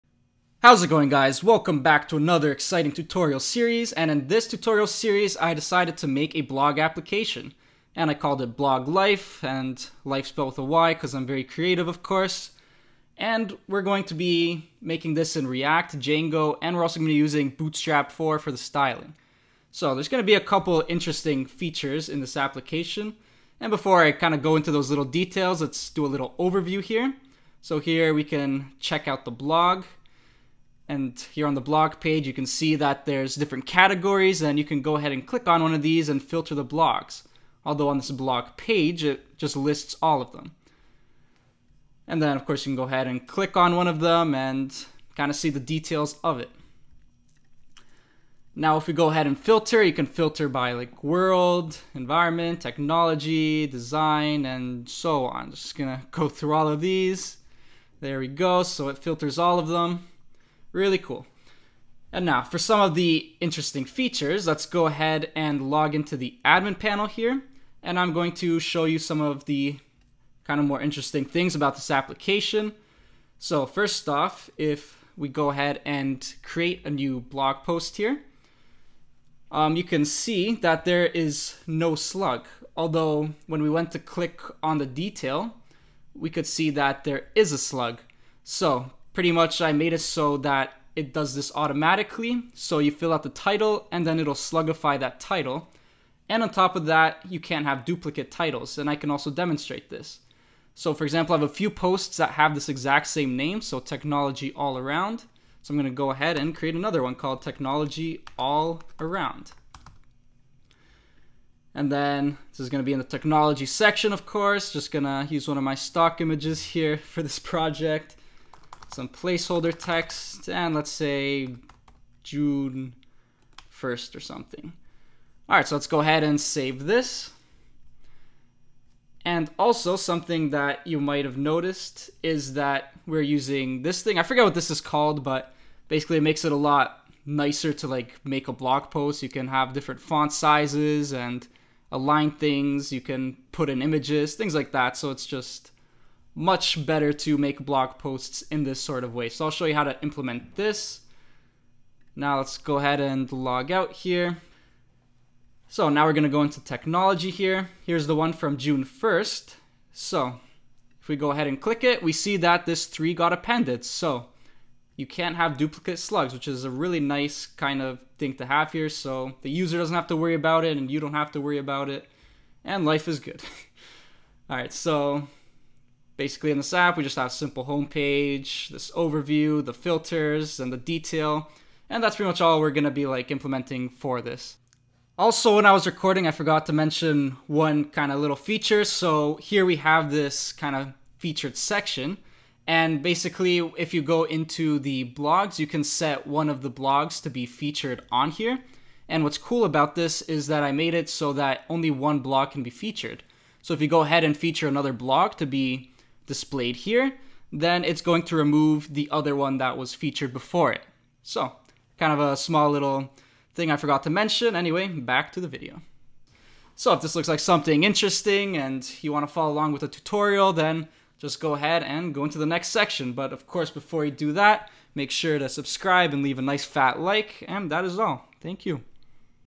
Django React Blog App | Part 1 - Overview
Hey, Bryan here. I share videos that help web developers up their development skills!

Today I'll be sharing the first part to a video series where I will be developing a full stack web application using React and Django with the Django Rest Framework. I will also be using Bootstrap 4 for the styling in the application.

This is going to be a blog application with a couple of interesting features implemented:
1. You will see how to implement a widget to create the blog post content which will be done using Django Summernote.
2. The blog posts will have a category, and in the application you will be able to filter the blog posts by category.
3. You will see how to have a slug automatically generated for you based on the title you provided.
4. You will see how to avoid having duplicate slugs so that you can even have posts in your application with the same title.
5. You will see how to have a featured blog that will be displayed at the top of the blog page.
6. You will see how to have it so that when you set another blog post to be featured, the previously featured blog post will be automatically removed from being featured.

Django Rest Framework will be used to make the backend REST API , and React will be used for making the frontend UI.

In this part, I will be doing an overview of the application we will be building and going over some of the different features and functionalities that will be implemented.